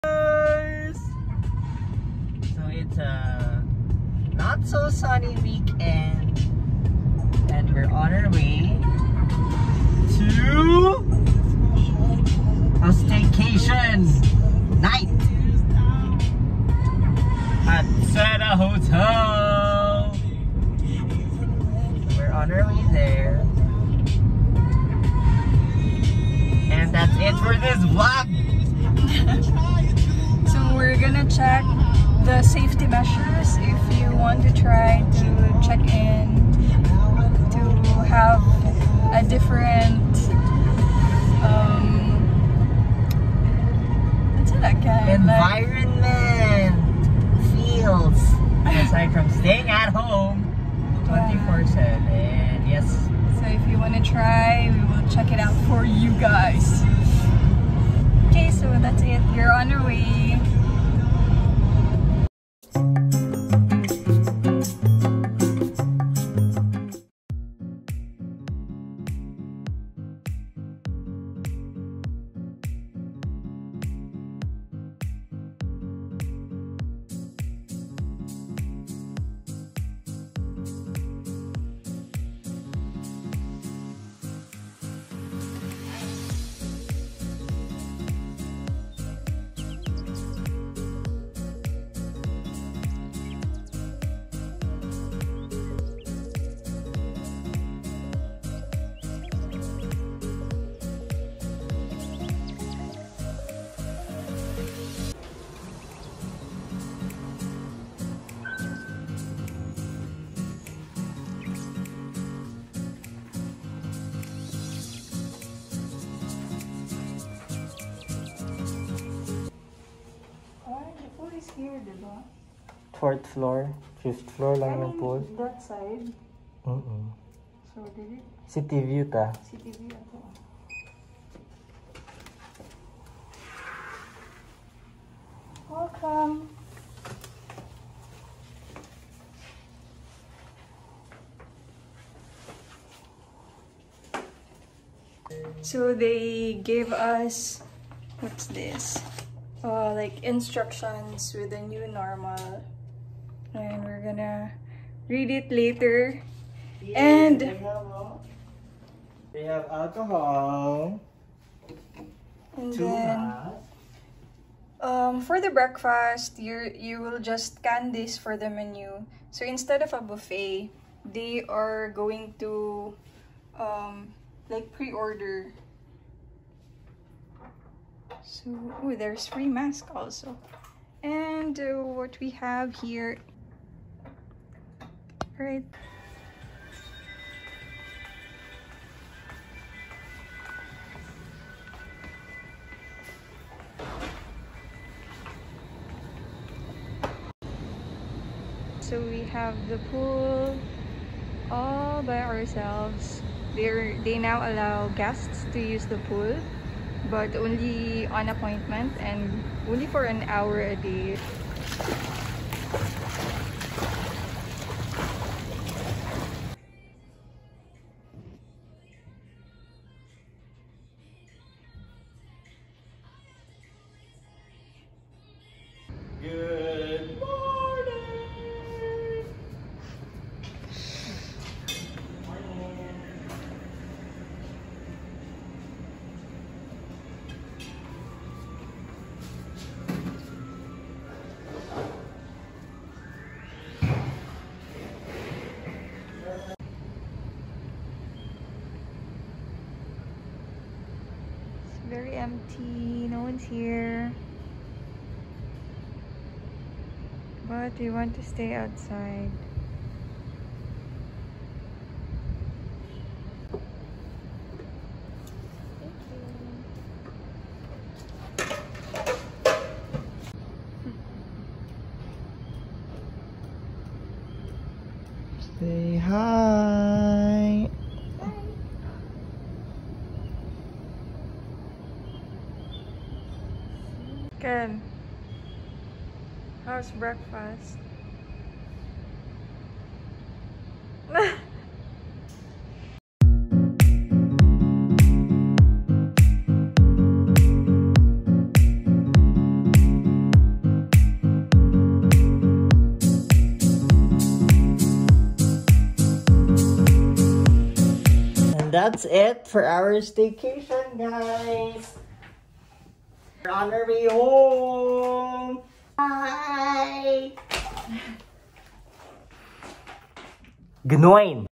Staycation in the New Normal | Seda Centrio | Philippines 2021
It has been almost a year since the pandemic started, a year since we're all stuck at home. By now, you probably wanted a change of environment but are still scared to go through all the travel requirements. If you're wondering how safe it is to do a staycation in the Philippines, watch this video! We stayed in Seda Centrio hotel in our city, Cagayan de Oro.